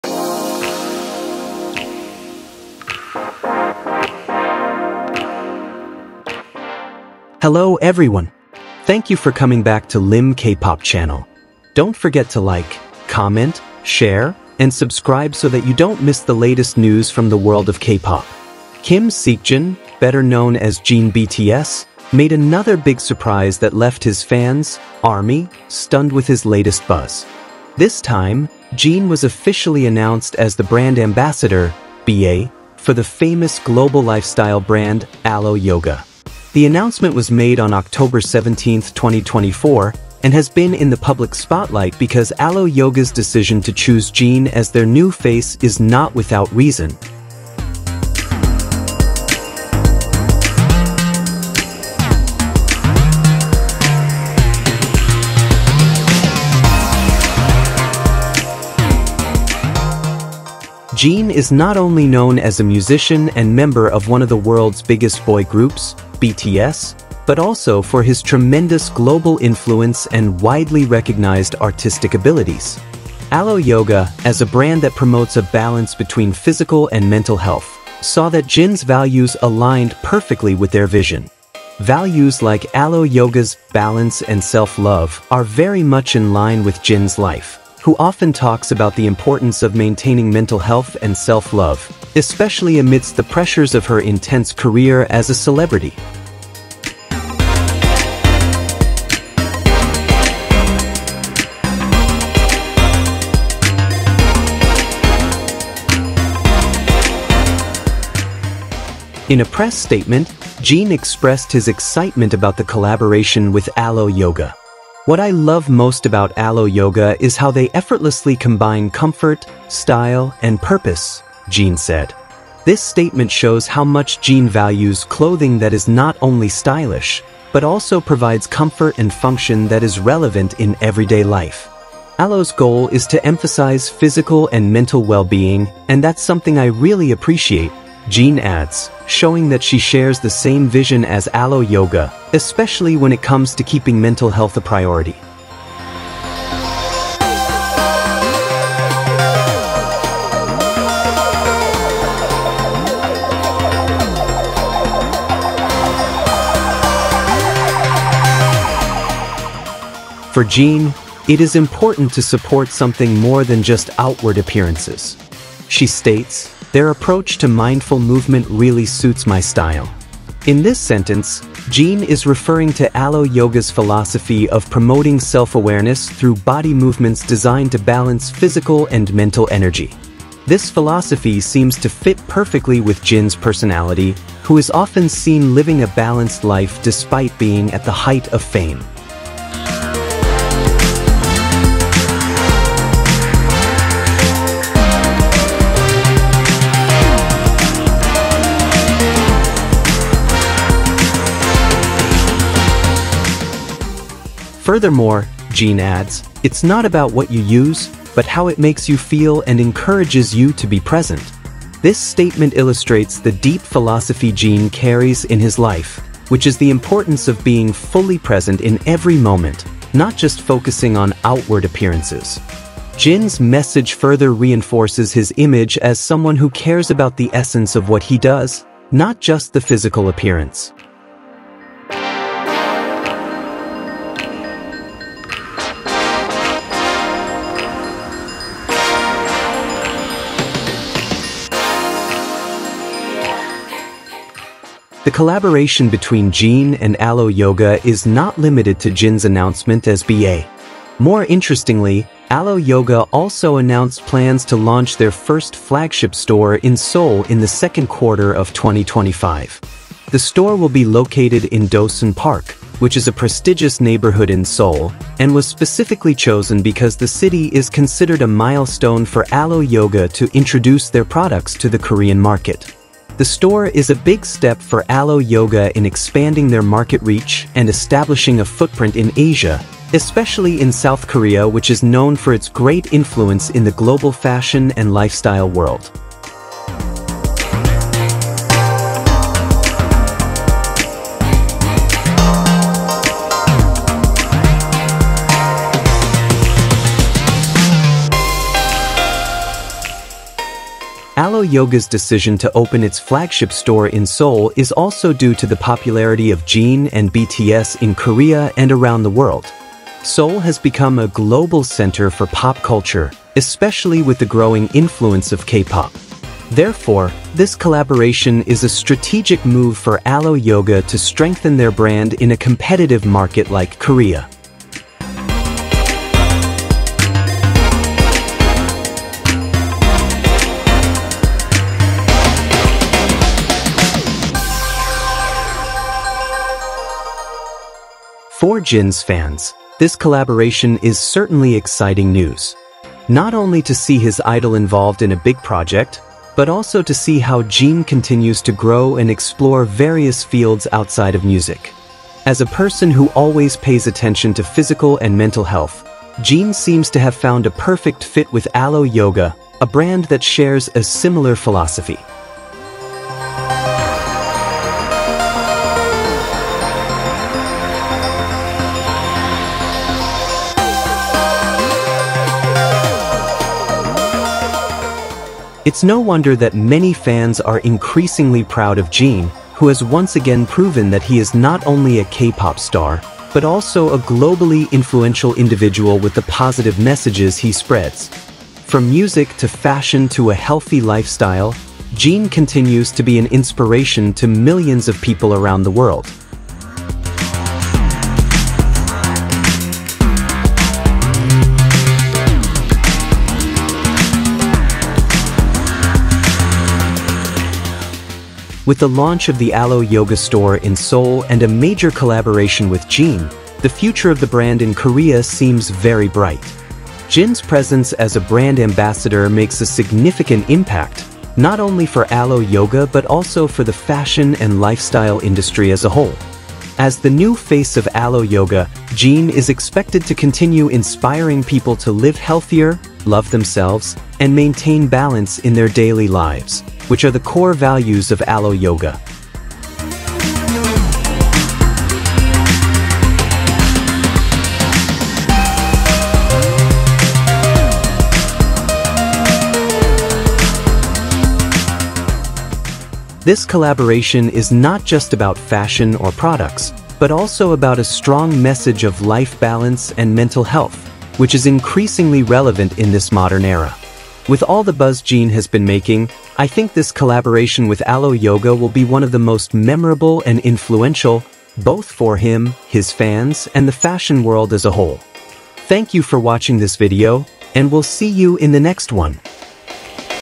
ARMY are shocked! Here's why Jin BTS was chosen as the brand's new BA
Kim Seokjin, better known as Jin BTS, made another big surprise that left his fans, ARMY, stunned with his latest buzz. This time, Jin was officially announced as the brand ambassador (BA) for the famous global lifestyle brand, alo Yoga. The announcement was made on October 17, 2024, and has been in the public spotlight because alo Yoga's decision to choose Jin as their new face is not without reason.